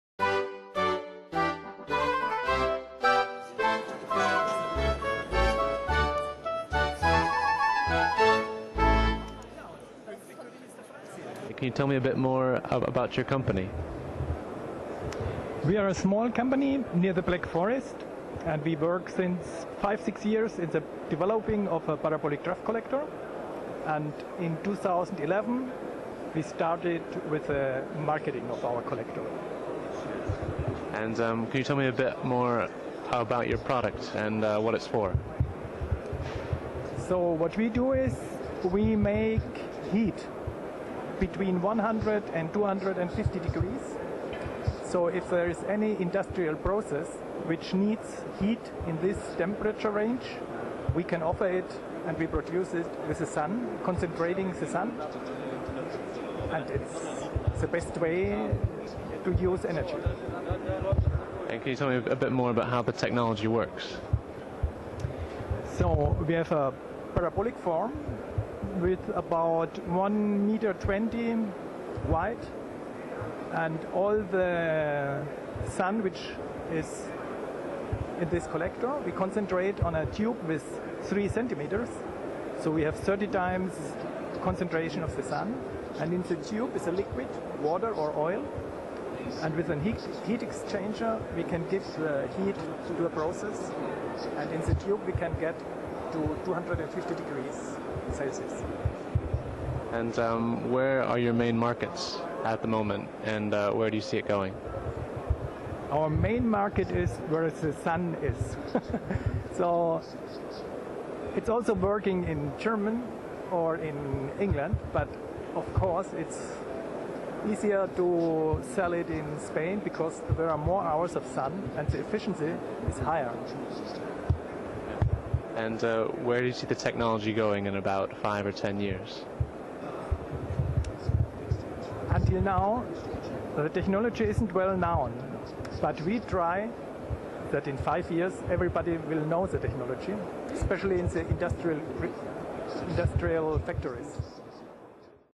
Smirro Interview: Concentrated Solar Thermal Heat
Smirro is a young, motivated company dedicated to the  issue of renewable energy. In addition to the development of their solar concepts, Smirro also operates an energy-efficiency consulting service.